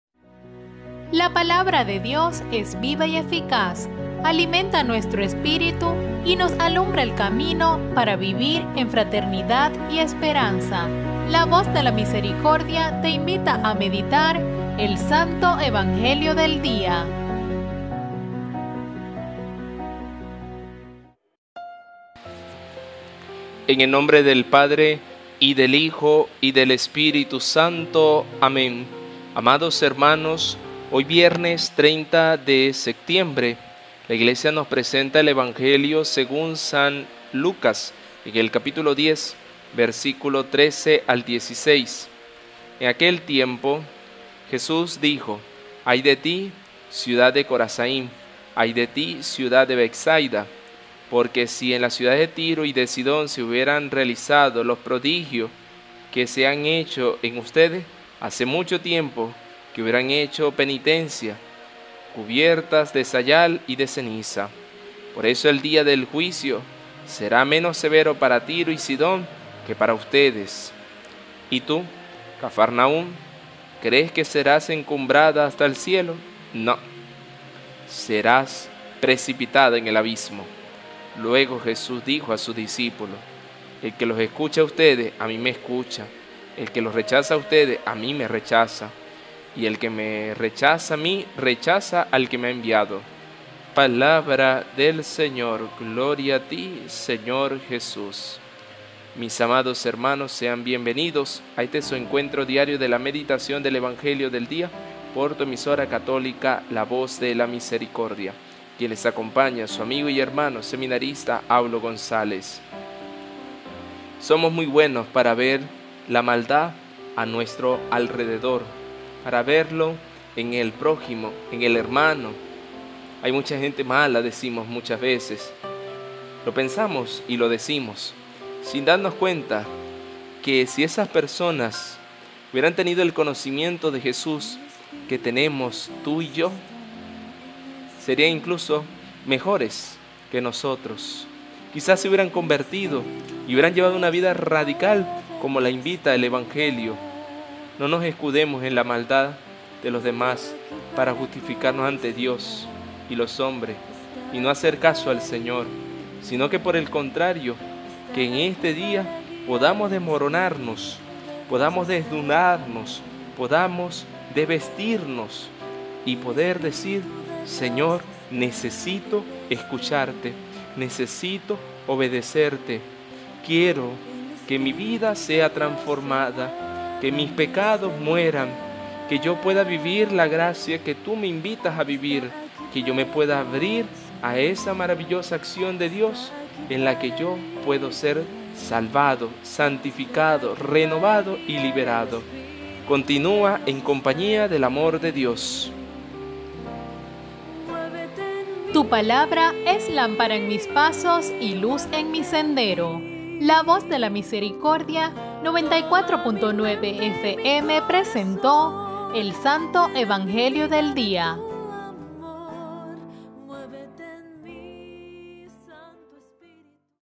Santo Evangelio del Día (30/09/2022) San Lucas 10, 13 - 16. Seminarista Aulo González.
San Lucas 10,13-16: Quien me rechaza a mí, rechaza al que me ha enviado.

En aquel tiempo, dijo Jesús:

-¡Ay de ti Corozaín, ay de ti, Betsaida! Si en Tiro y en Sidón se hubieran hecho los milagros que en vosotras, hace tiempo que se habrían convertido, vestidos de sayal y sentados en la ceniza.

Por eso el juicio les será más llevadero a Tiro y a Sidón que a vosotras.

Y tú, Cafarnaún, ¿piensas escalar el cielo? Bajarás al abismo.

Quien a vosotros os escucha, a mí me escucha; quien a vosotros os rechaza, a mí me rechaza; y quien me rechaza a mí, rechaza al que me ha enviado.